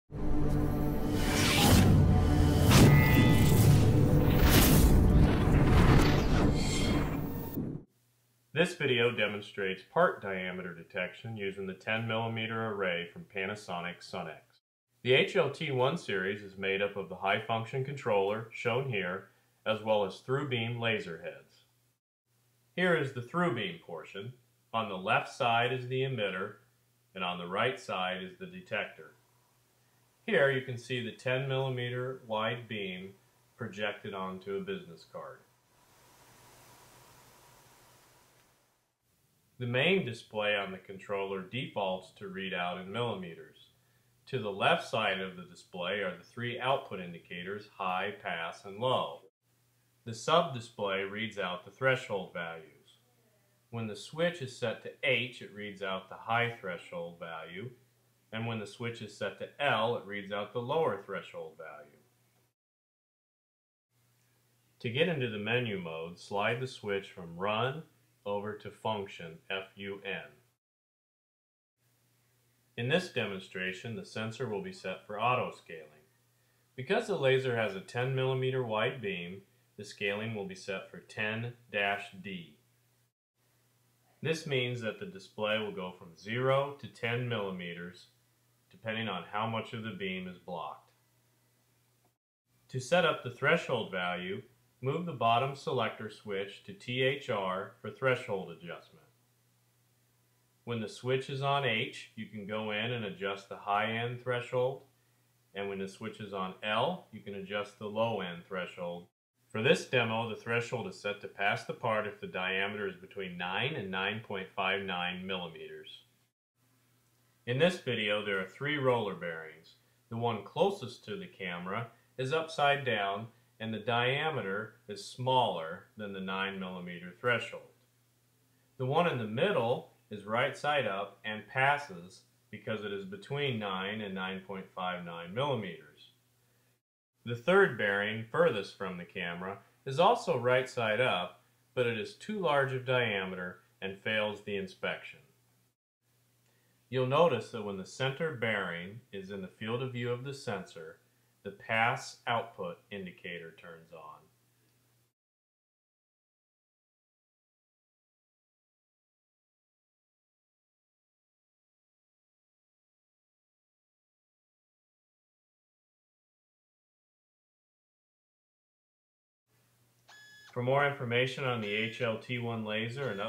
HL-T1 Series Laser Sensor
The HL-T1 series of laser collimated beam sensors combines extremely compact sensor heads with a high-functionality intelligent controller. Three sensor heads are available from the long-range, small beam width HL-T1001(A/F) to the wide beam HL-T1010(A/F With a 4-micron resolution and an 8-micron minimum object diameter, the HL-T1 series offers incredibly high precision for positioning and measurement applications. An optional side-view attachment provides ease and simplicity in mounting the sensor head. The HL-T1 controller features an easy to see dual digital display with a convenient four-key layout for simple control. Three types of teaching functions have been integrated including: single level, two level and fully automatic teaching. Using the optional HL-AC1-CL calculation unit, two sensors can be connected together to enable addition or subtraction functions. This is perfect for applications such as sheet width measurement. The analog output is selectable between current or voltage and HI/LO/GO outputs are integrated as well (available as PNP or NPN).